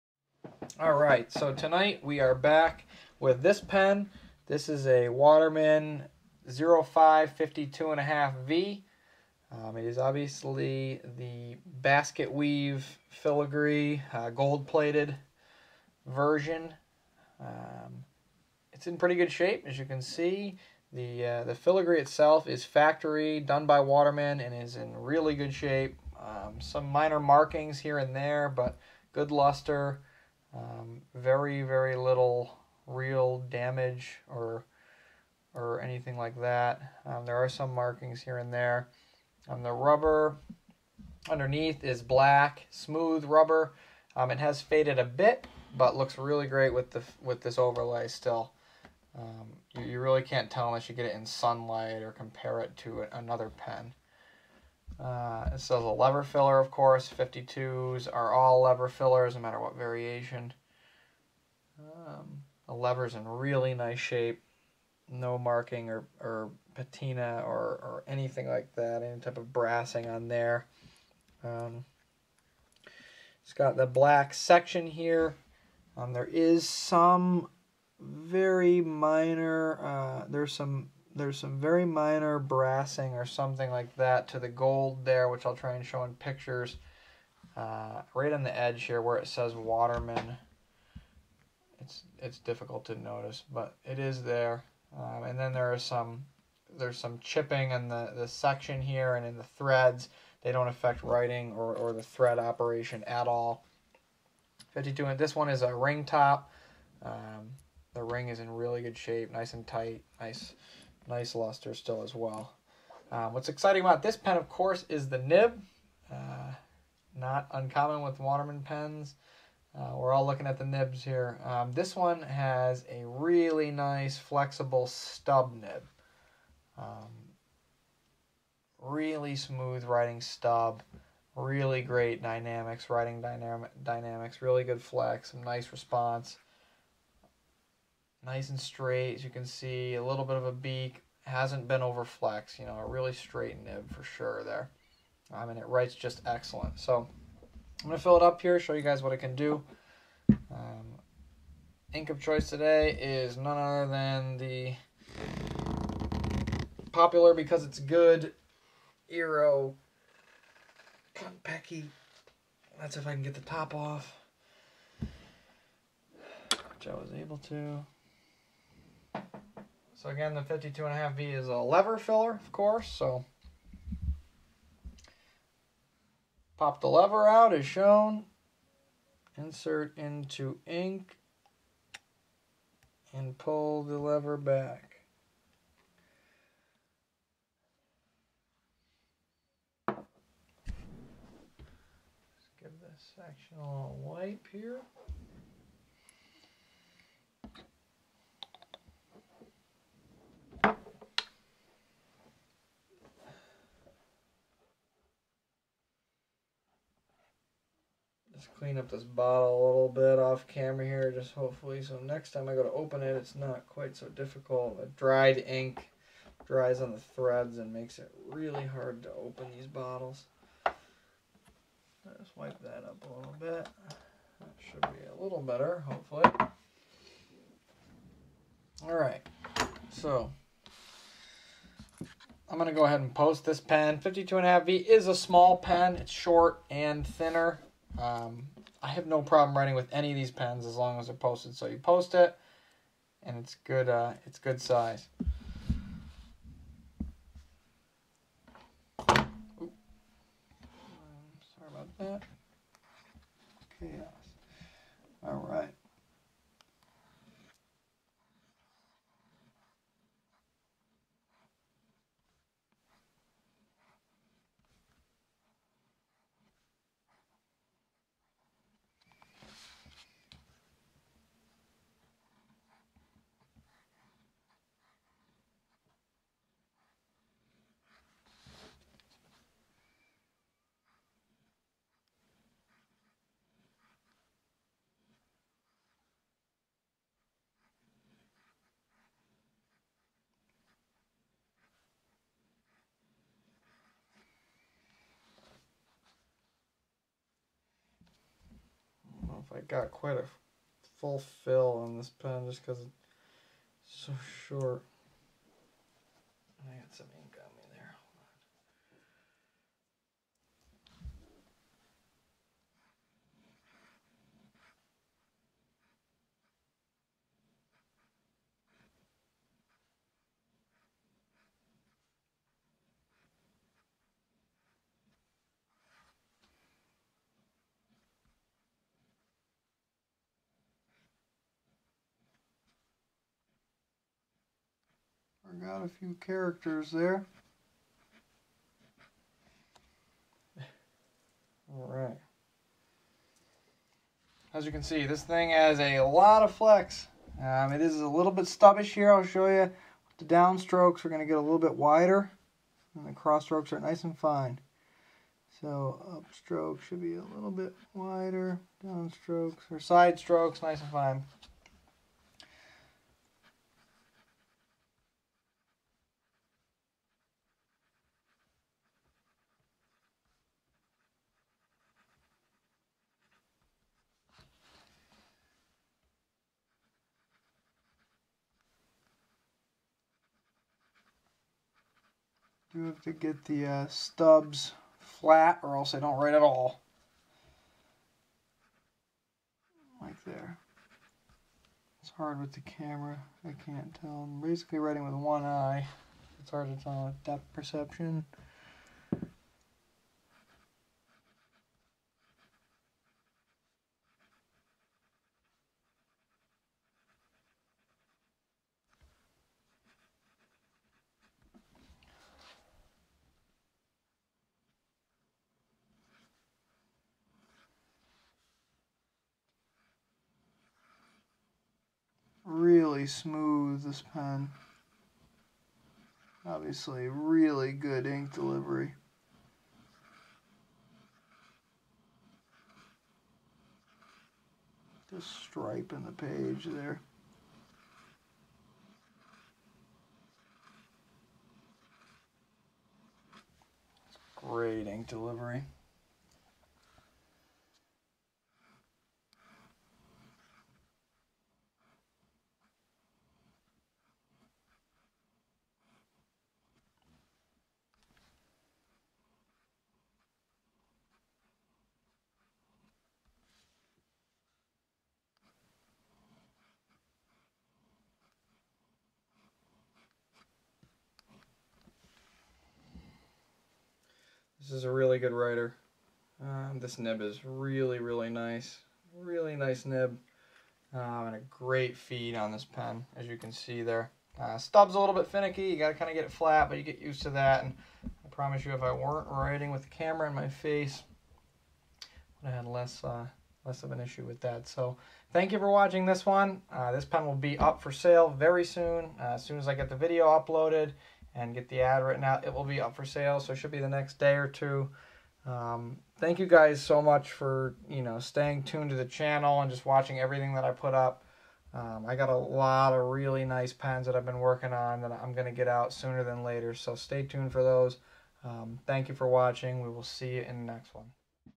Waterman 0552 1/2V Gold Overlay SOFT FLEXY Stub
This is a gorgeous Waterman 0552 1/2V with quite the stub nib. Beautiful gold-filled overlay. Check out the nib on this one...